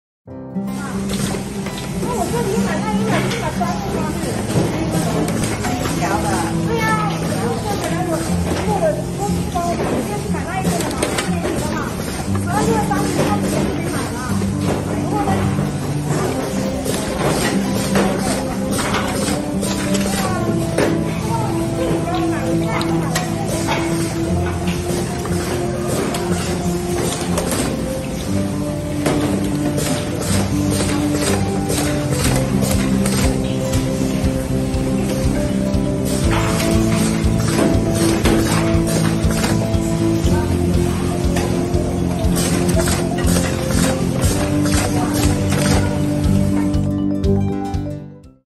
Desktop Liquid Filling Process for Juice, Water, Milk, and Oil
MAKWELL's  Automatic Desktop Liquid Oil Filling Machine With Conveyor Beverage Juice Filler equipped with 4 Filling heads with an automated filling machine. It can fill 40-60 bottles per minute with 0.5% filling precision. Suitable for beer, milk, water, oil, juice.